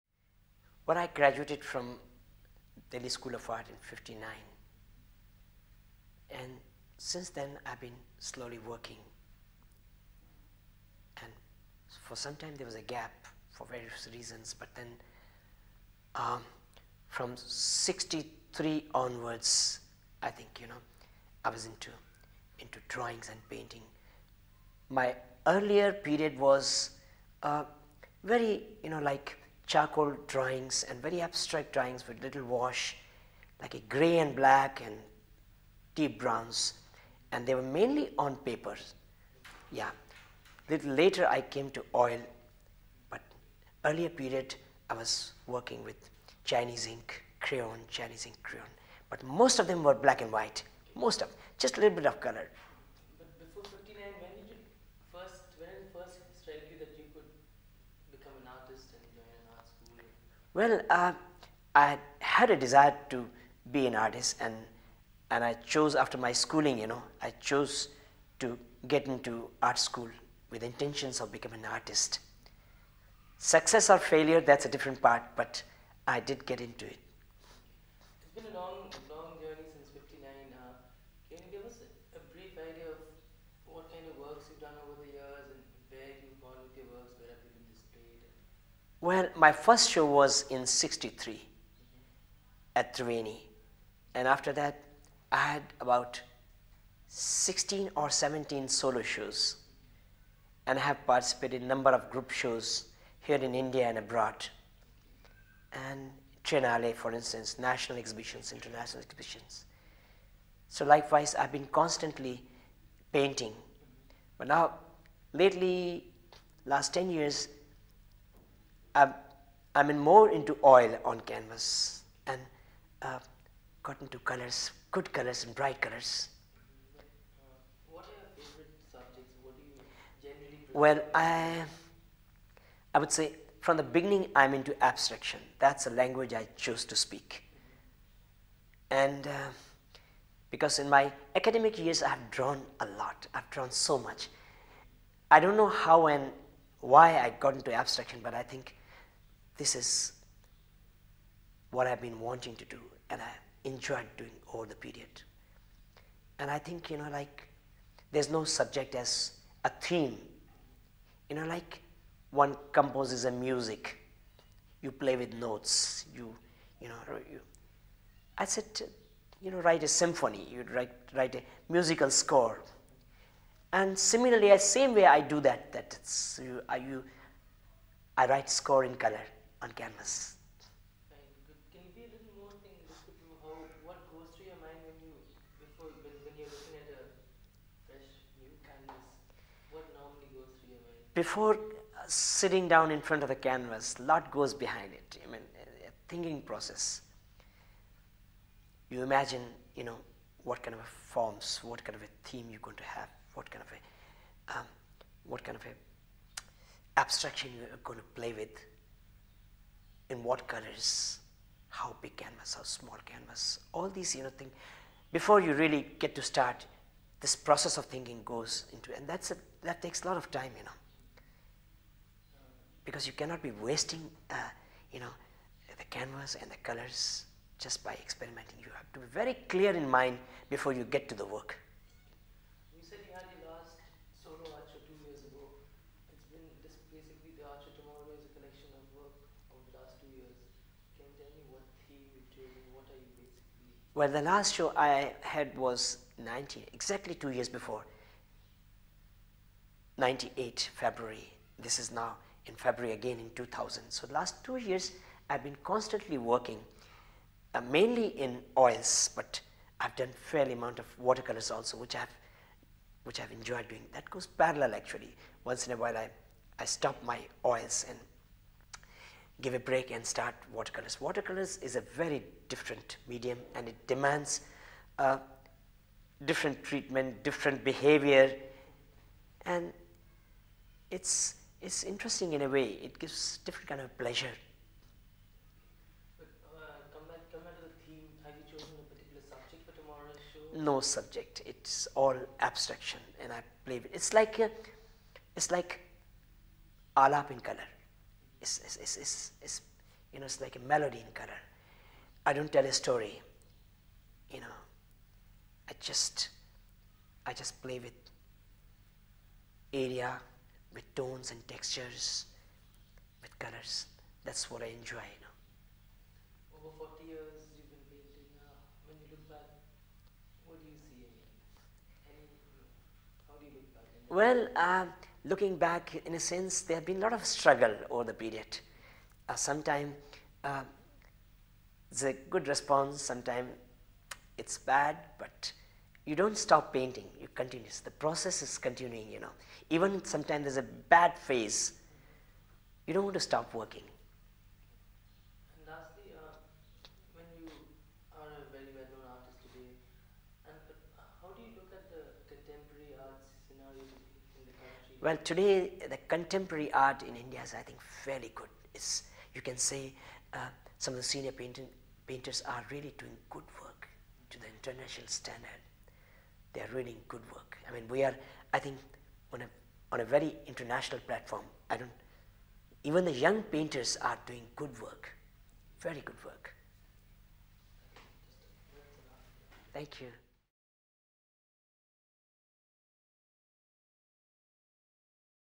"My first exhibition was at Triveni in 1963": Gopi Gajwani
Gopi Gajwani says "When I graduated from Delhi School Of Art in 1959, since then I have been slowly working. For some time there was a gap for various reasons but then from 1963 onwards, I was into drawings and painting. My earlier work included charcoal drawings and abstract drawings with little wash. Like grey, black and teak browns, mainly on paper. Later I came to oil. But earlier I was working with Chinese ink and crayons. But most of them were black and white with few colours. Well, I had a desire to be an artist and I chose after schooling to get into an art school to become an artist. Success and failure is a later part."

Gopi Gajwani is an Indian artist. Gopi Gajwani has had several gallery and museum exhibitions, including at the Chawla Art Gallery, Square One Mall and at the Visions Art.